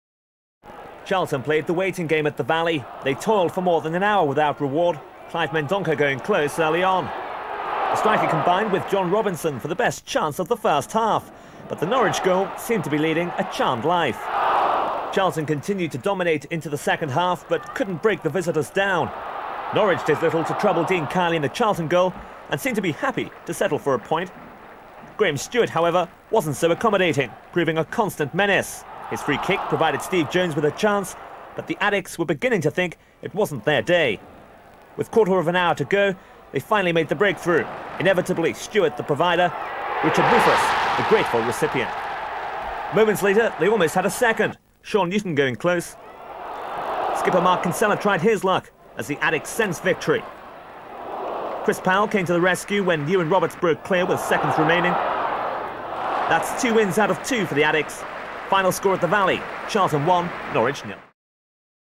1999/00 Charlton v Norwich City (Highlights)
21st August 1999 at The Valley (19,639)
Charlton Athletic 1 (0)(Rufus 73): Kiely, Brown, Youds, Rufus, Powell, Newton, Kinsella, Stuart, Robinson, Hunt (S. Jones 60), Mendonca (Pringle 86). Subs. (not used) Ilic, K. Jones, Salako.
Norwich City 0 (0): A. Marshall, Sutch, Mackay, Fleming, Fuglestad, Anselin (Diop 72), Mulryne (L. Marshall 62), de Blasiis, Llewellyn (Coote 77), Roberts, Dalglish. Subs. (not used) Green, Carey.
Referee: Dermot J. Gallagher (Banbury).
Booked: Roberts (31), deliberate handball; Dalglish, foul on Kiely.